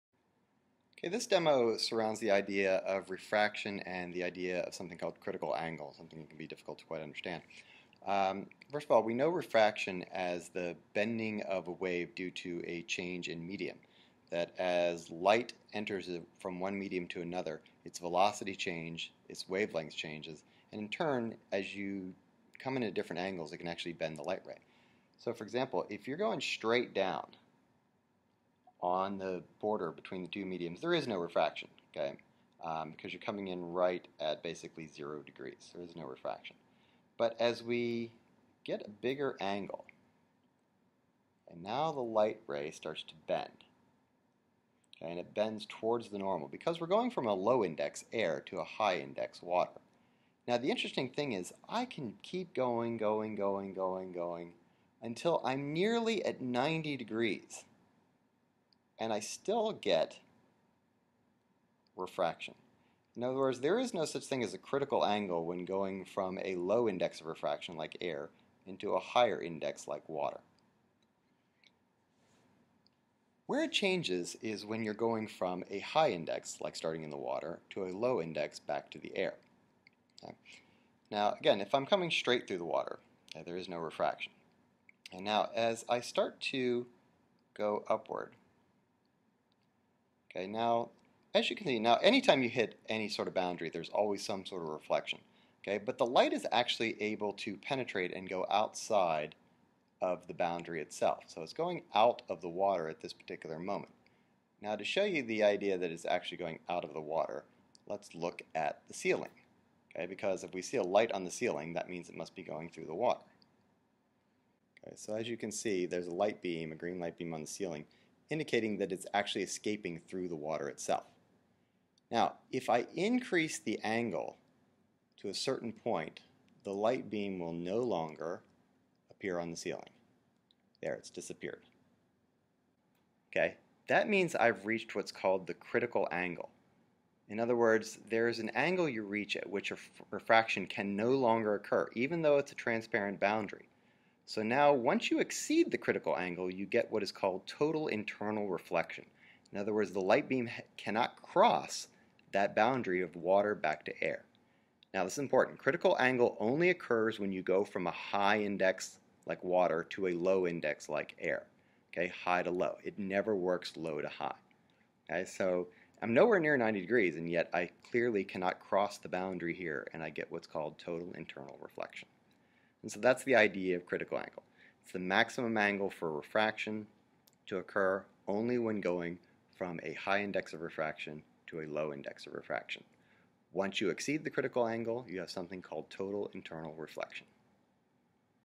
Critical Angle
This is an example of the refraction of light, particularly related to the idea of critical angle. The critical angle is the maximum angle at which light will refract, but only occurs when light travels from a medium with a high index of refraction (like water, n = 1.333) to a medium that has a lower index of refractions (like air, n =1).